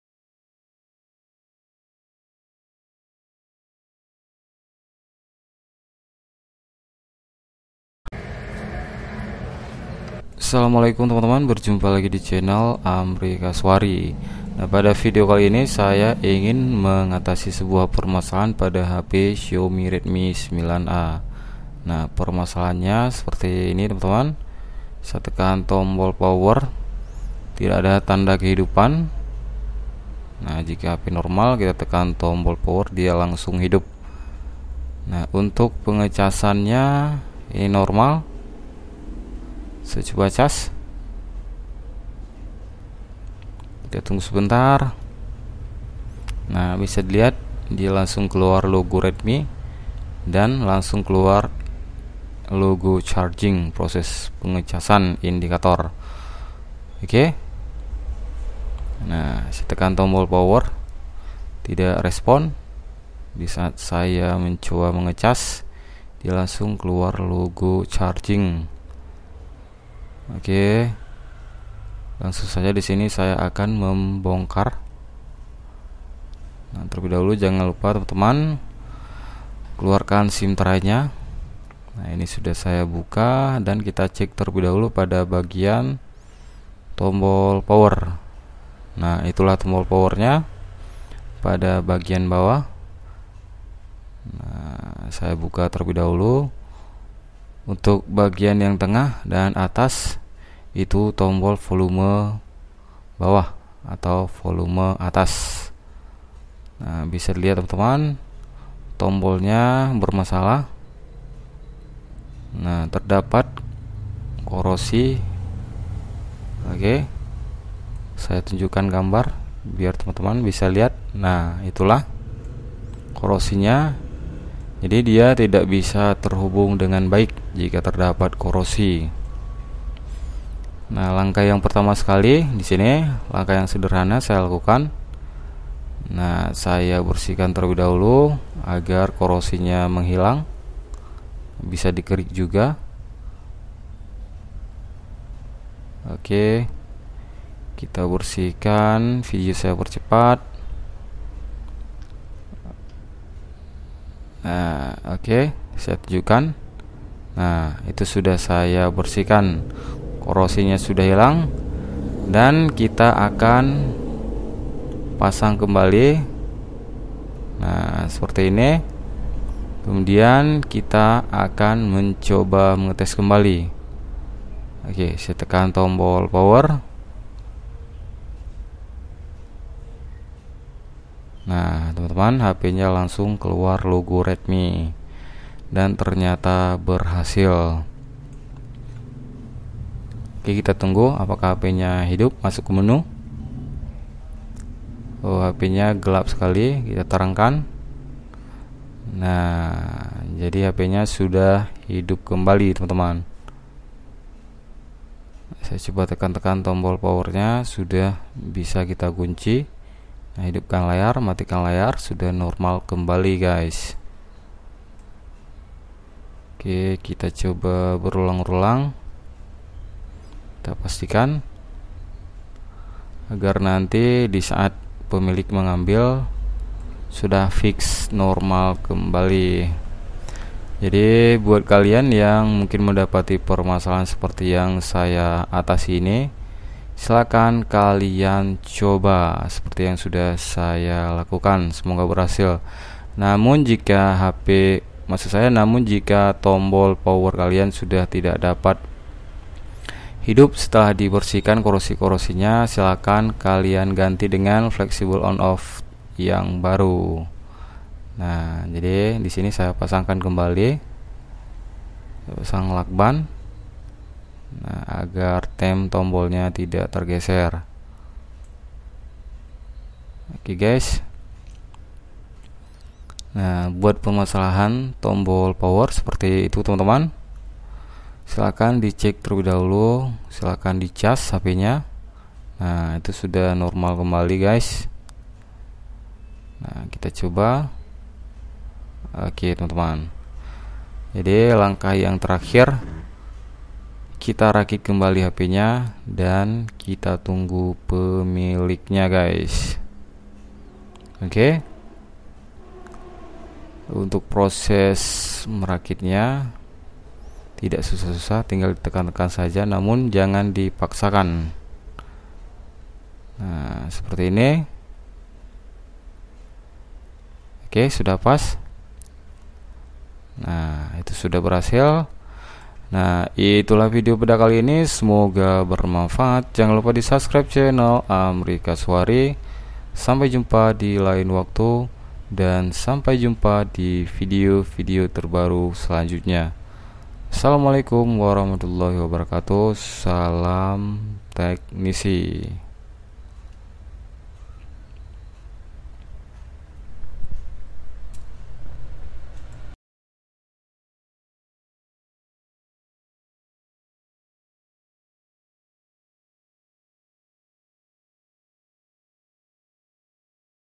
Perbaiki xiaomi redmi 9a mati (Tanpa ganti Spare Parts) di rumah bisa..?👍
Perbaiki xiaomi redmi 9a mati tanpa ganti spare parts dirumah bisa
semoga bermamfaat buat semuanya ya gan.